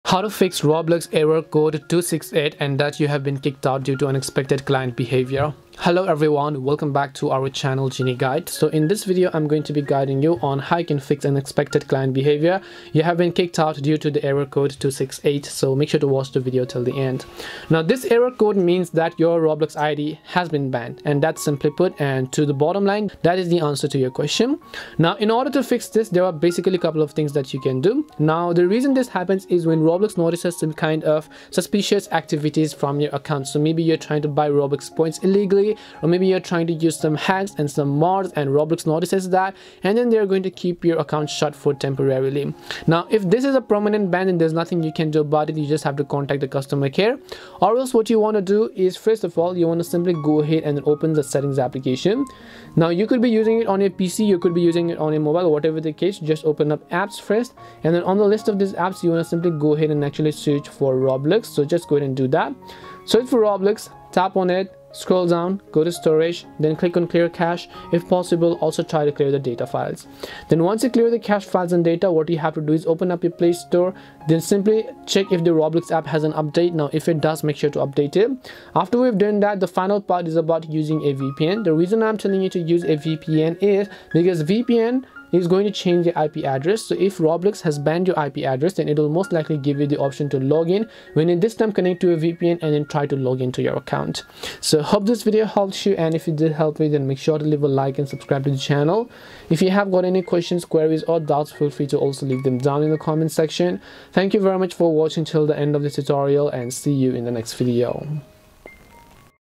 How to Fix You Have Been Kicked Due Unexpected Client Behavior Roblox Exploiting Error Code 268!!
In this video, I will show you how to fix you have been kicked out due to unexpected client behavior Roblox Disconnected Error Code 268!!.

~ Chapters:
0:00 Introduction
0:18 Why This Happens
0:25 What You Can Do
1:35 Conclusion